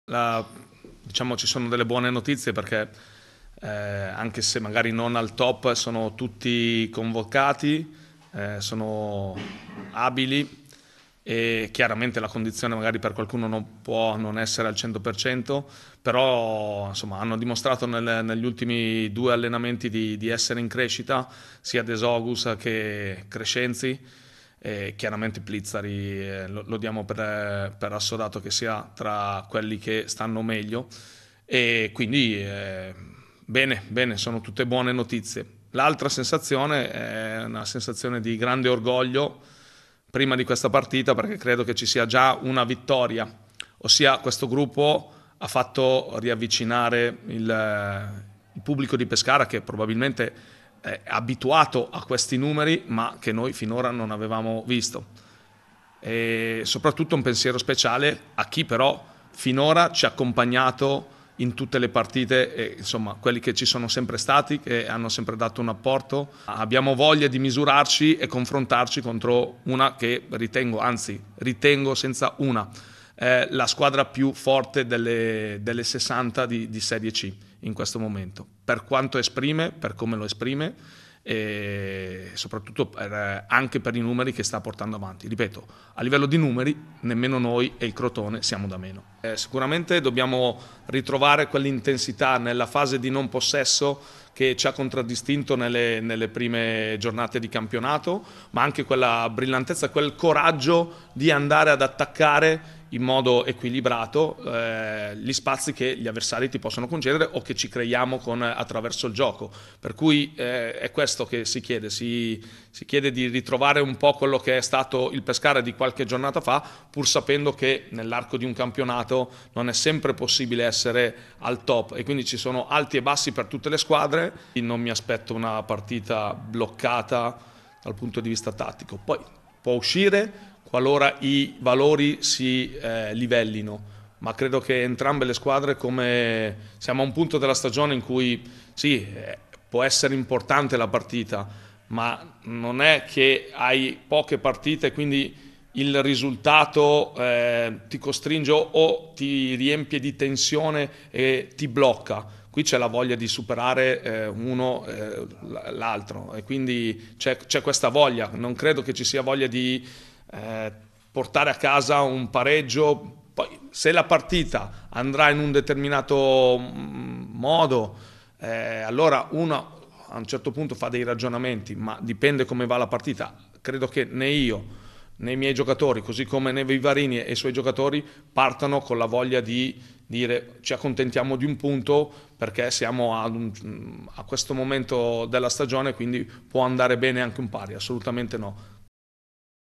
Pescara, Colombo: "Voglia di misurarsi con la più forte della Lega Pro"
Vigilia di campionato per il Pescara che domani, in un Adriatico-Cornacchia che torna ad ospitare il grande pubblico, affronta il Catanzaro primo della classe. Tutti a disposizione per Colombo che ammette come ci sia curiosità di misurarsi contro la formazione più forte delle 60 della Serie C.